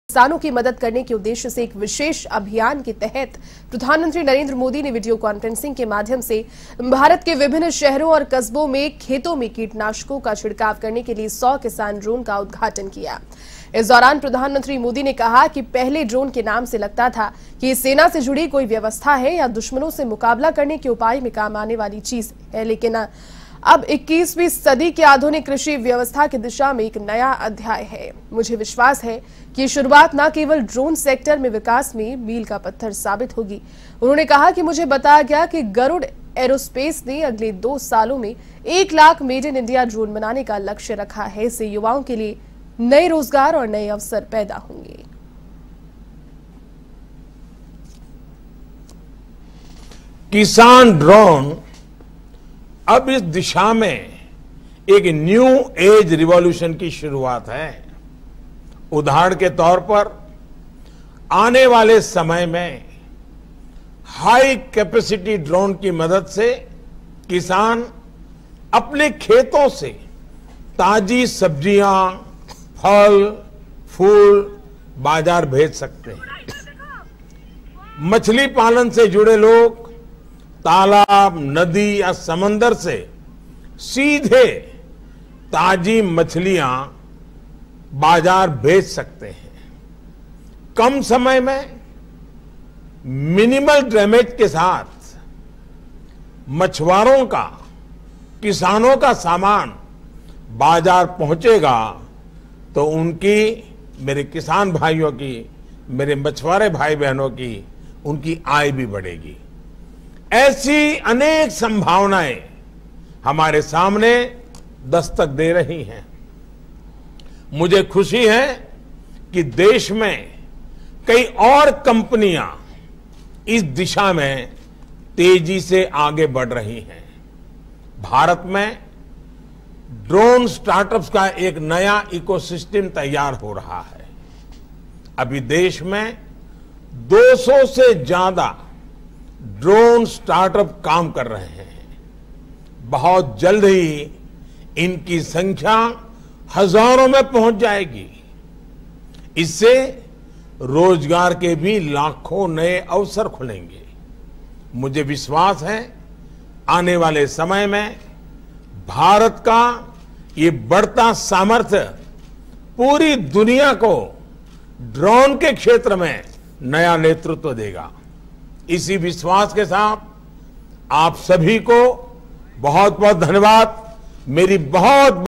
100 शहरों कस्बों के खेतों में उड़े ड्रोन पीएम मोदी ने किया उद्घाटन, बोले- कृषि क्षेत्र का नया अध्याय
100 शहरों कस्बों के खेतों में उड़े ड्रोन पीएम मोदी ने किया उद्घाटन, बोले- कृषि क्षेत्र का नया अध्याय